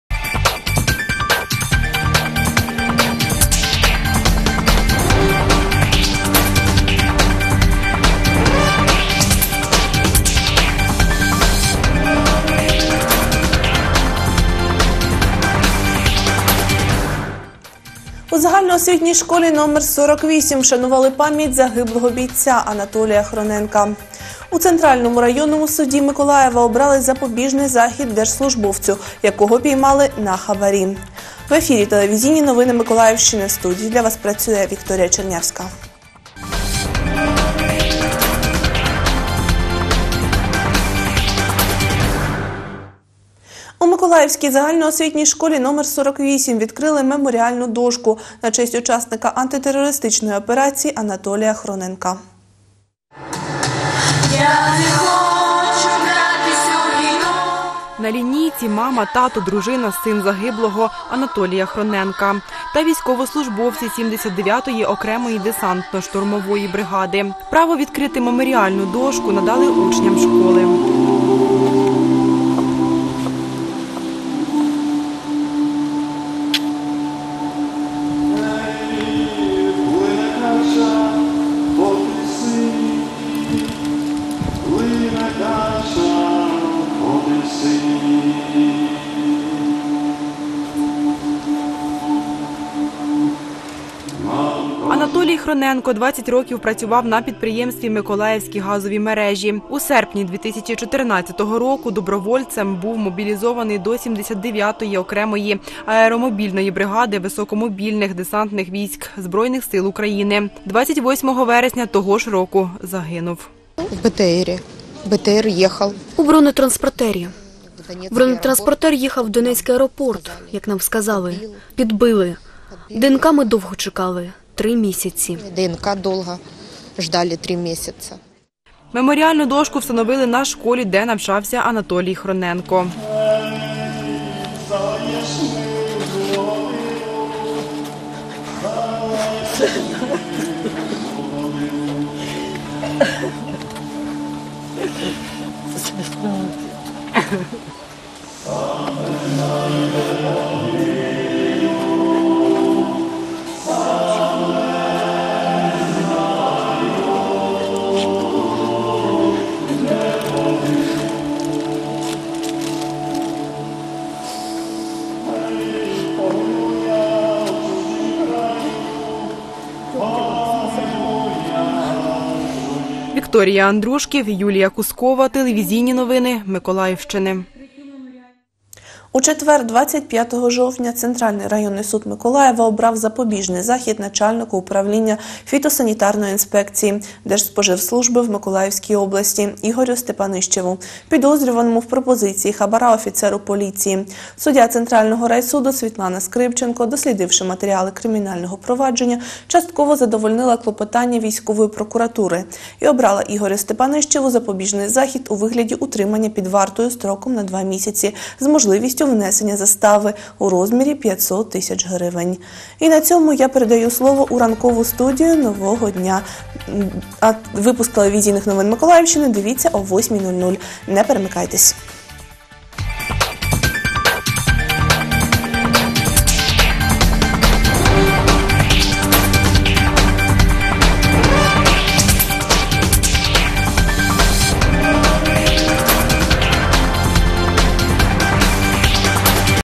26.10.2018. Новини. 07:30
Інформаційна картина дня (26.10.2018) поглядом кореспондентів телеканалу “Миколаїв” (випуск новин — 07:30):
- у загальноосвітній школі №48 вшанували пам’ять загиблого бійця Анатолія Хроненка;
- у Центральному районному суді Миколаєва обрали запобіжний захід держслужбовцю, якого піймали на хабарі.